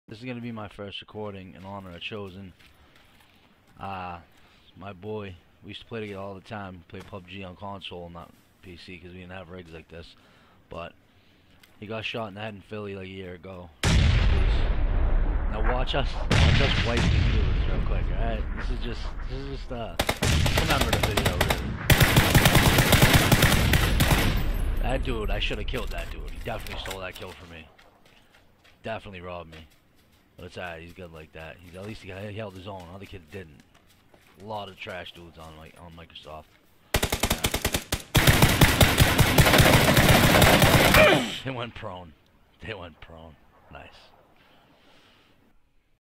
Me and my boy Chris aka Chosen1 cleaning up the last few scrubs
This was one of the last games before my boy passed away he was only 20 way too young rest in piece my dude